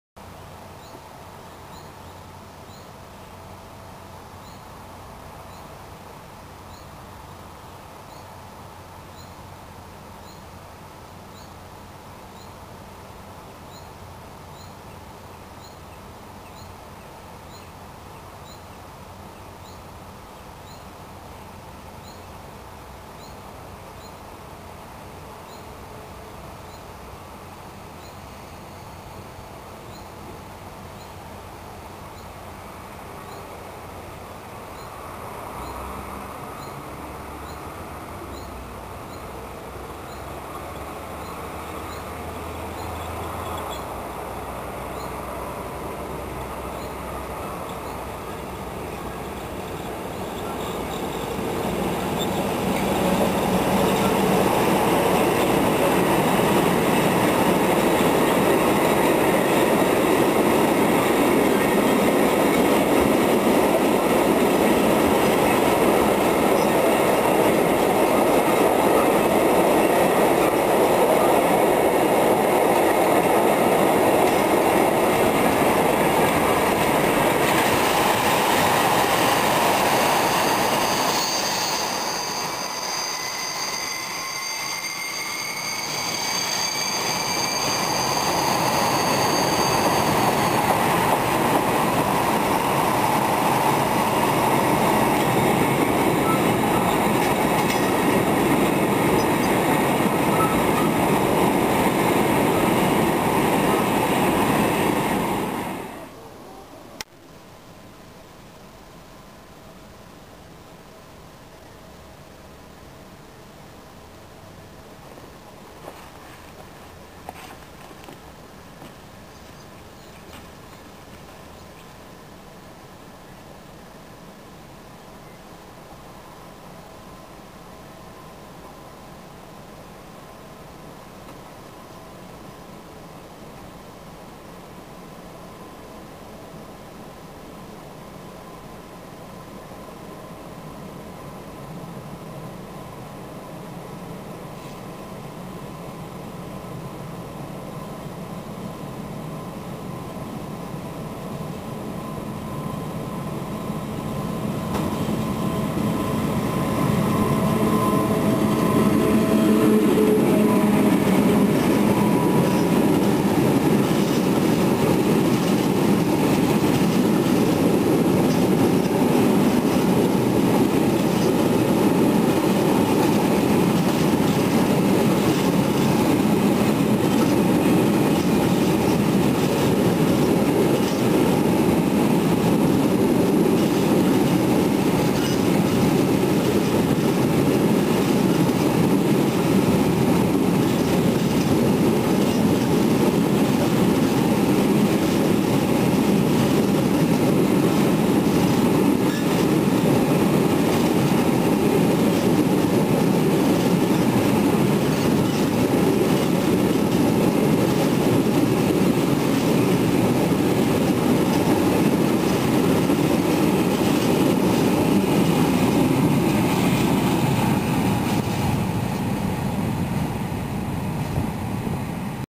dwa pociągi towarowe
ET22-932 i  ET22-863 wczesnym porankiem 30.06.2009 godz 5:12-5:32 Zielona Góra od strony Czerwieńska